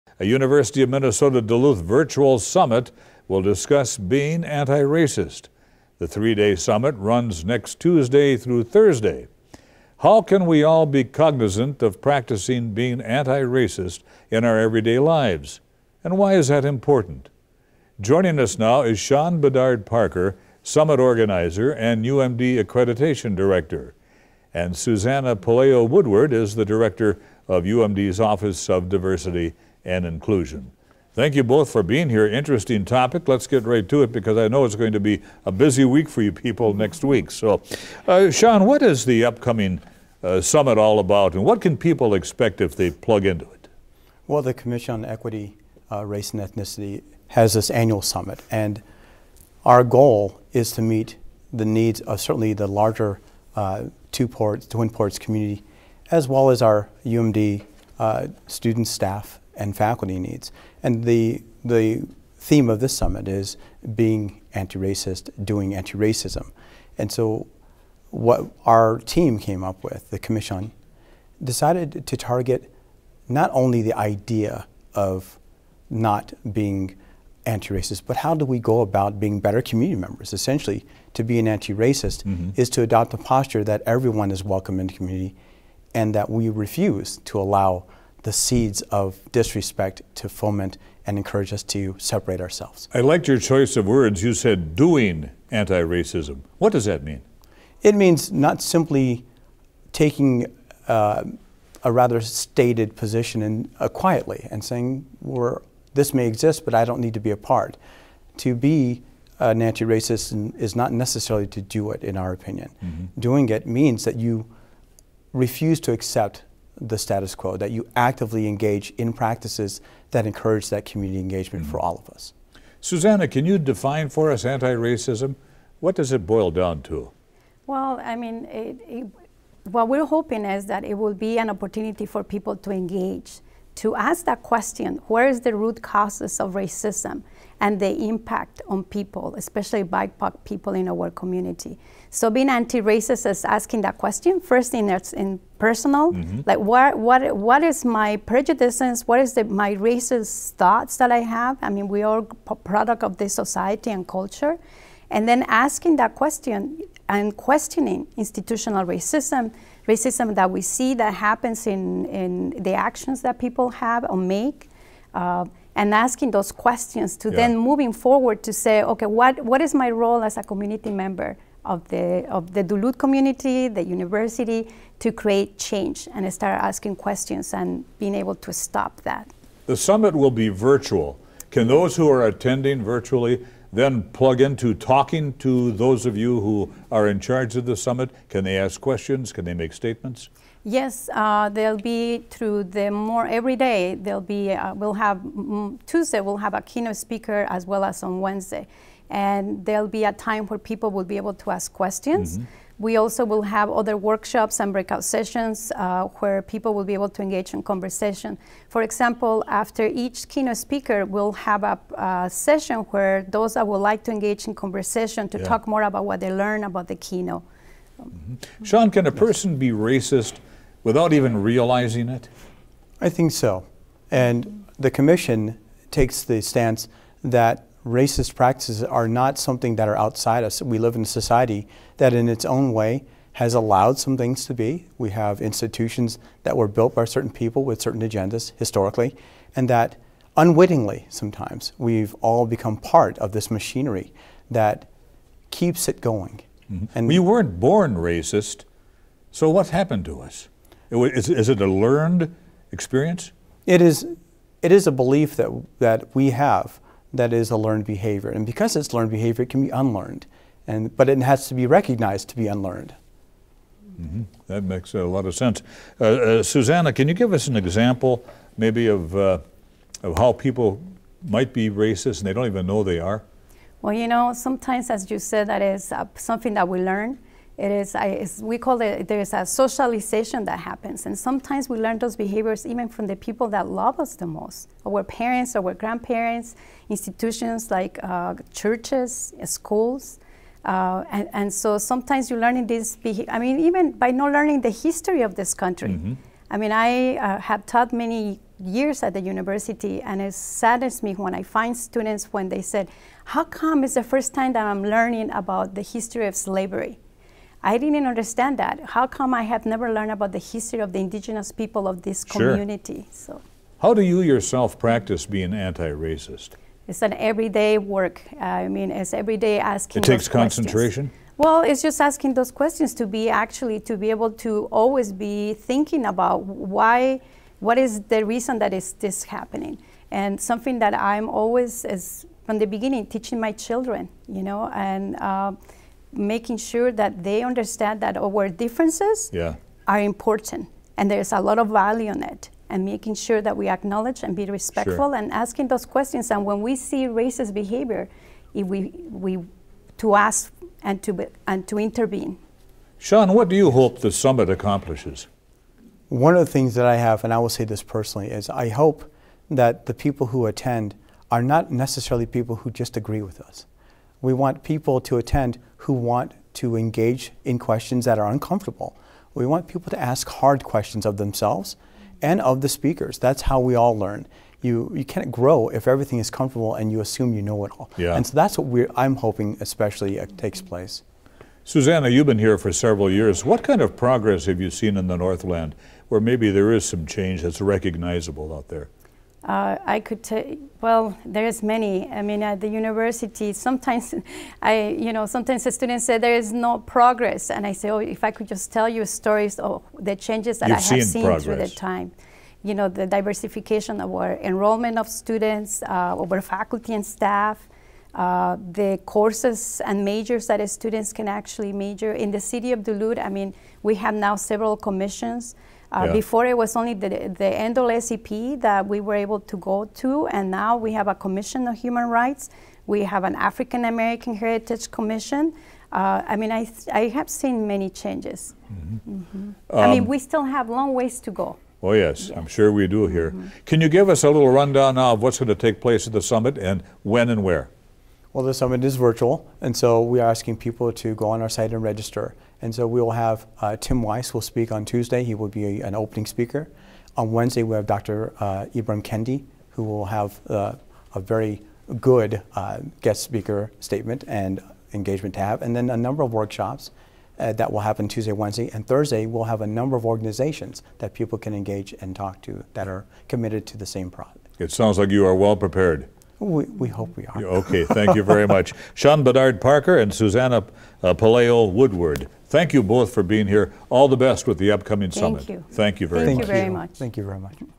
UMD Antiracism Summit
The University of Minnesota Duluth (UMD) is hosting a virtual summit next week addressing antiracism. The summit will teach the community how we can be cognizant of practicing antiracism. We were joined by summit organizer and Accreditation Director Sean Bedard-Parker as well as the Director of UMD's Office of Diversity and Inclusion Susana-Pelayo Woodward.